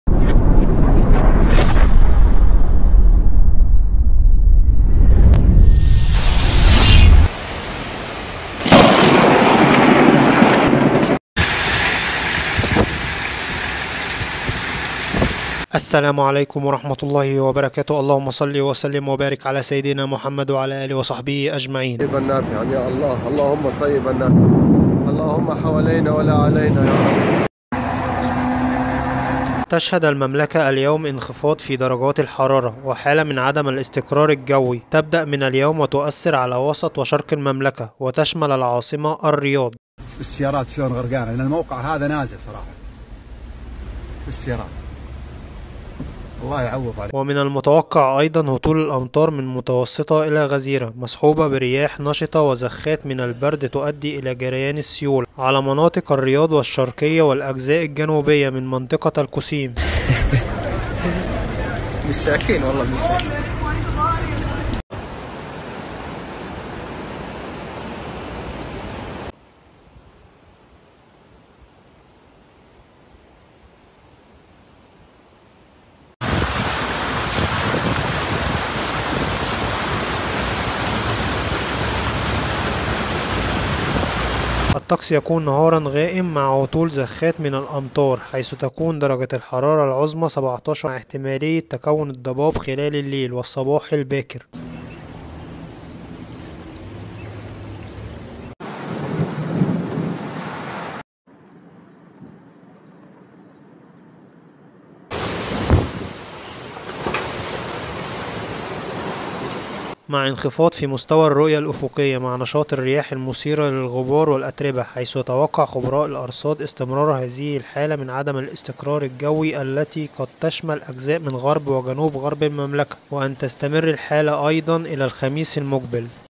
السعودية تتحول إلى محيط! فيضانات مفاجئة وأمواج كبيرة و إعصار رهيب في الرياض
تشهد المملكة اليوم انخفاض في درجات الحرارة وحالة من عدم الاستقرار الجوي تبدأ من اليوم و توثر علي وسط وشرق المملكة وتشمل العاصمة الرياض ومن المتوقع هطول الأمطار من متوسطة الي غزيرة مصحوبة برياح نشطة وزخات من البرد تودي الي جريان السيول علي مناطق الرياض والشرقية والاجزاء الجنوبية من منطقة القصيم الطقس يكون نهارا غائم مع هطول زخات من الأمطار حيث تكون درجة الحرارة العظمي 17 درجة مع احتمالية تكون الضباب خلال الليل والصباح الباكر مع انخفاض في مستوى الرؤية الأفقية مع نشأط الرياح المسيرة للغبار والأتربة  حيث يتوقع خبراء الأرصاد استمرار هذه الحالة من عدم الاستقرار الجوي التي قد تشمل اجزاء من غرب وجنوب غرب المملكة وان تستمر الحالة الي الخميس المقبل..